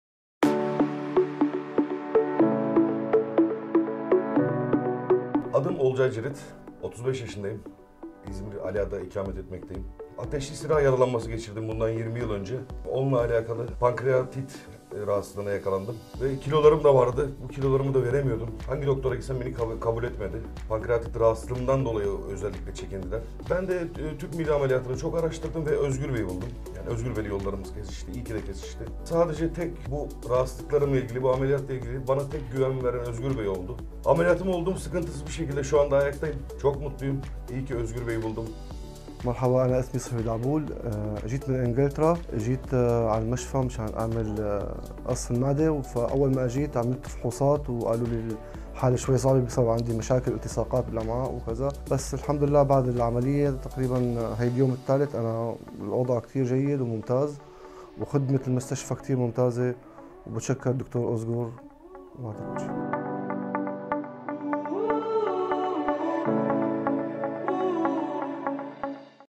Gastrıc Sleeve | Olcay's Journey
Our patient, Mr. Olcay said goodbye to his excess weight by coming to Ekol Hospitals. He had been suffering from an illness for a long time, which caused him to put on weight. He is very pleased to meet Özgür Kavak, one of the surgeons at Ekol Hospital. We wish our patient the best health.